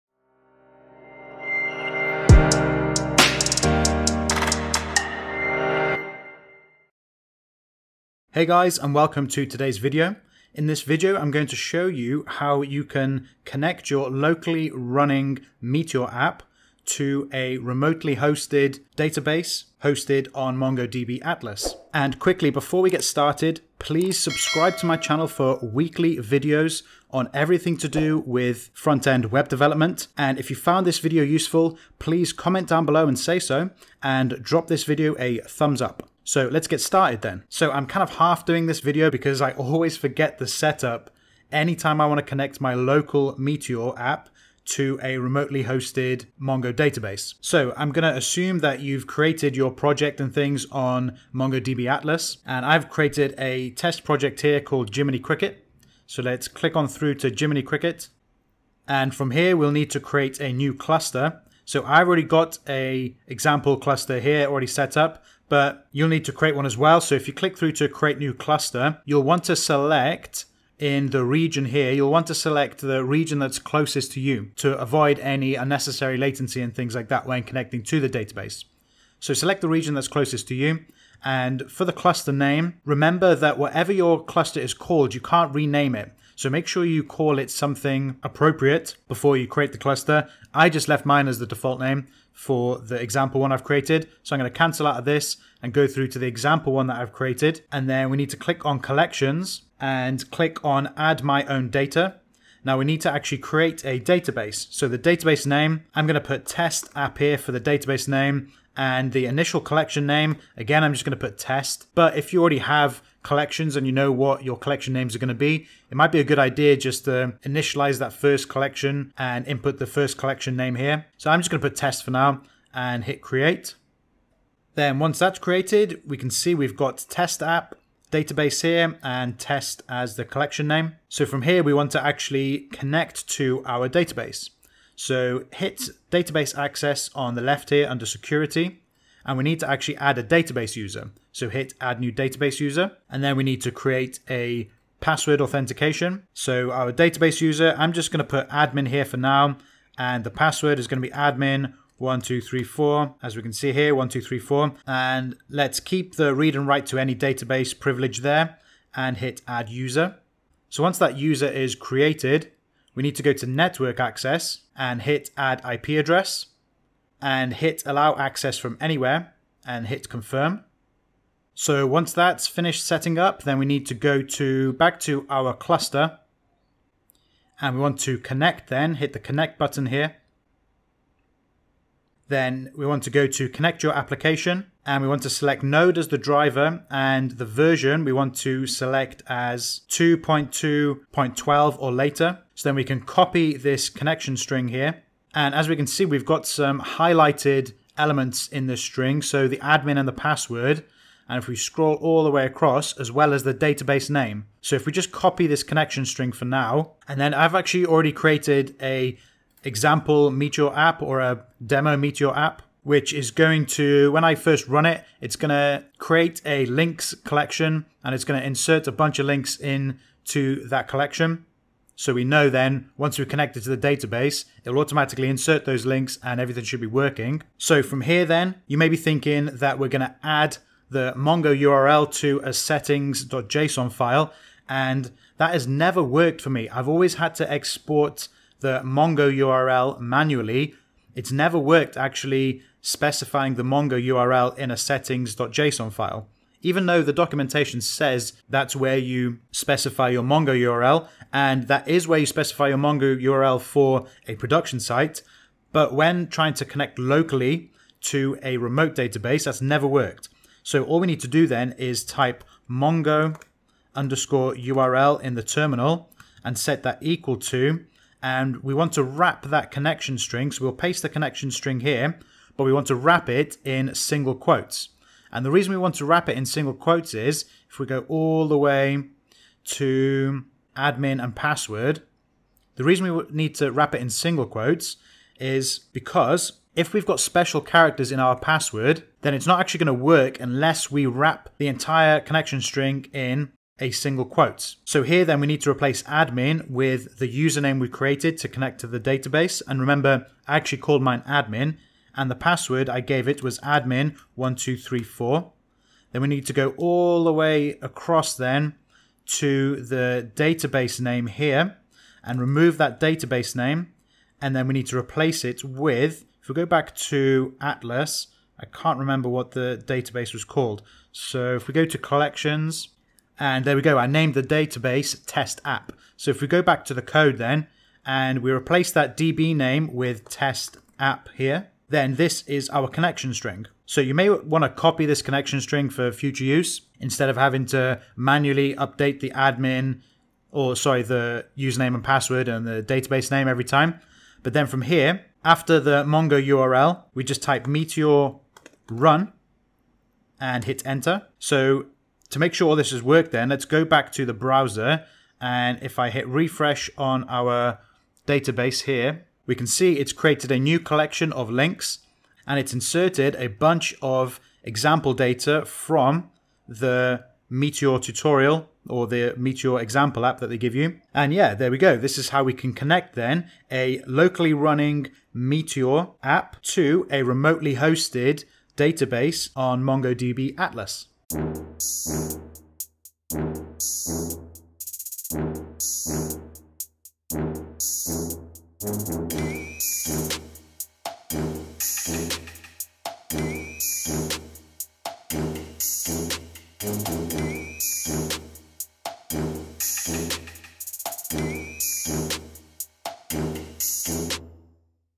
Connect localhost Meteor JS to remote MongoDB Atlas (2020 tutorial)
🎓 MeteorJS tutorial on how to connect your locally running (localhost) Meteor JS application to a remotely hosted Mongo database on MongoDB Atlas. Meteor JS in 2020 is still relevant 😉
Follow this meteor mongodb tutorial if your mongo_url environment variable on localhost is not working or not applying.